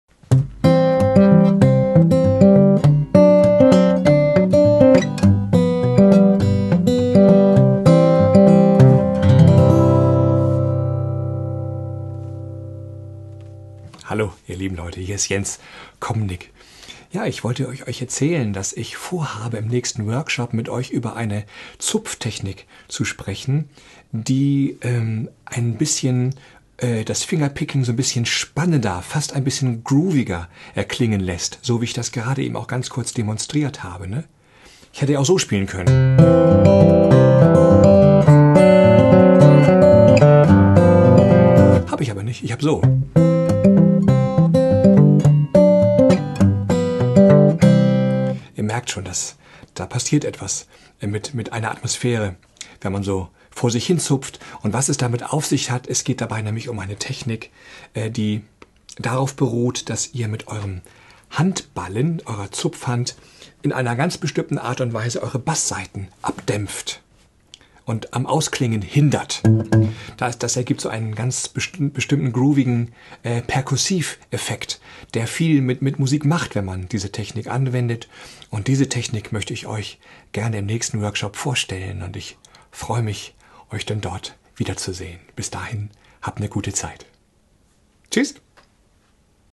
Acoustic Player 2-2019, Workshop Fingerstyle Basics von Jens Kommnick
Ein wichtiges Stilmittel, um Spannung und klangliche Abwechslung in einem Fingerstyle-Arrangement zu erreichen, ist die Spieltechnik "palm mute": das leichte Abdämpfen der Tone mit dem Handballen. Der entstehende Klang ist perkussiv und markant, dies erzeugt eine besonders spannende Atmosphäre und kernigen Groove. Jens Kommnick zeigt die praktische Anwendung dieser Spieltechnik im Stil von Gitarristen wie Werner Lämmerhirt und Doc Watson.